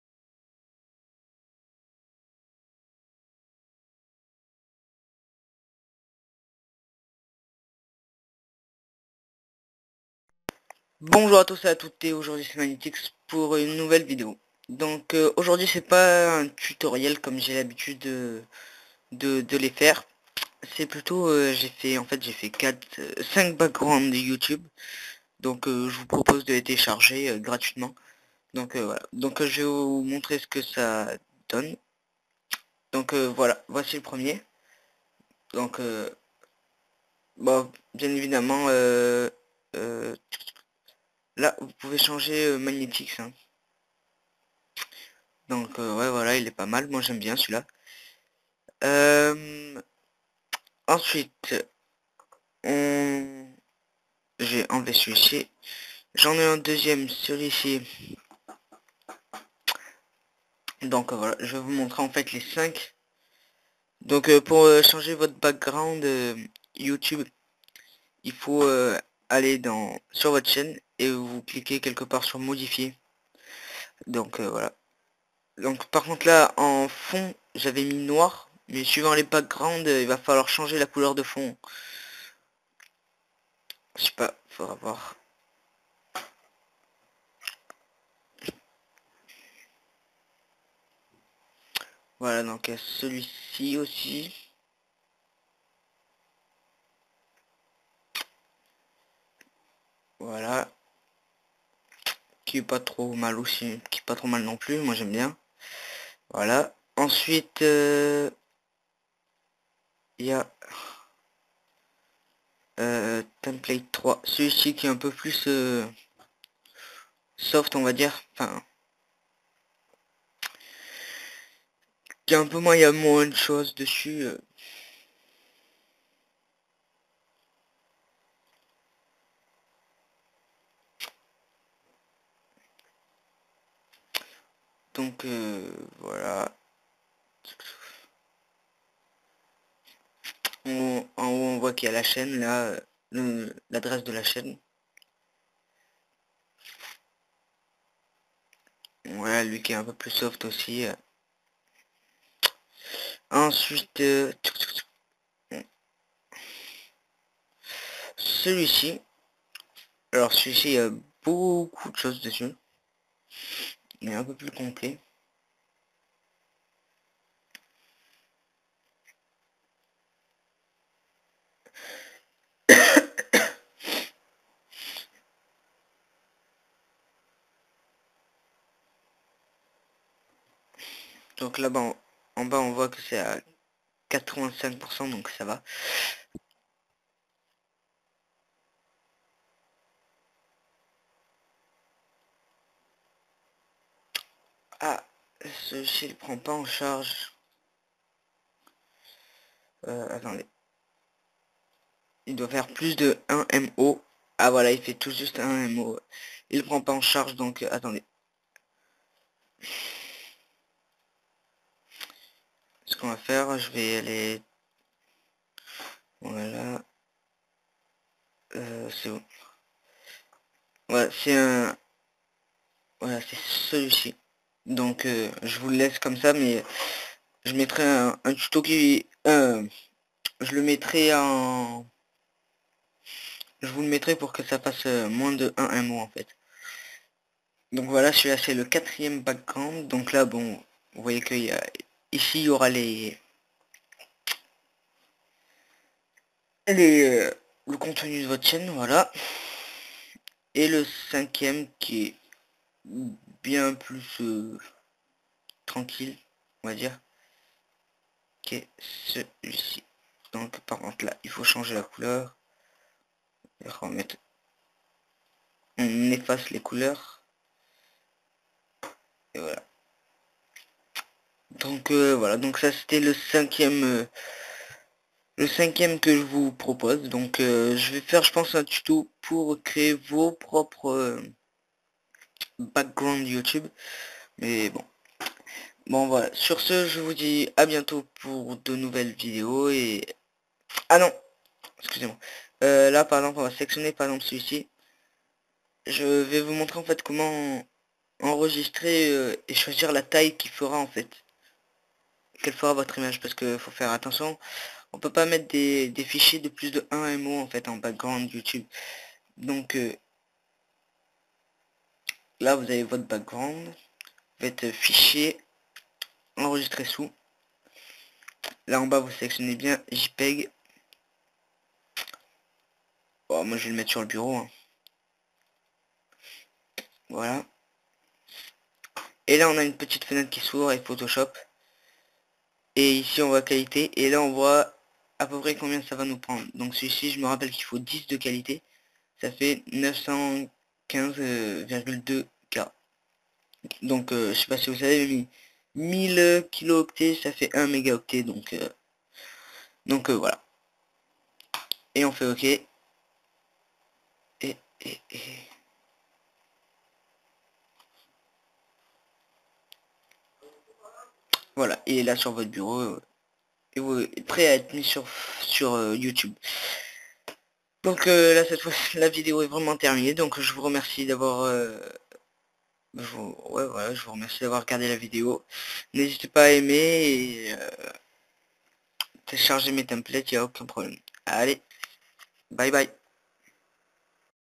5 templates de background Youtube
Aujourd'hui j'ai décidé de vous faire 5 petits templates pour vos background YT ! Je vous les donne gratuitement avec le fichier .psd pour le modifier a volonté :)

Donc merci à tous ceux qui regardent cette vidéo et s'il vous plait, cliquez sur "J'aime" et Abbonez-vous: